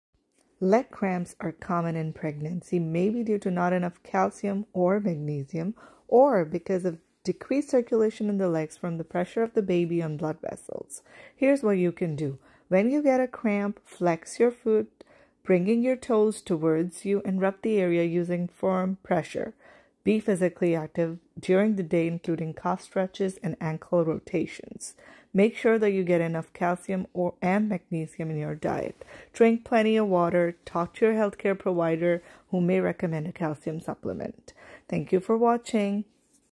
Pregnancy Discomforts: Leg cramps
Leg cramps are a common discomfort faced by women during pregnancy. You can find some tips to deal with leg cramps in this video.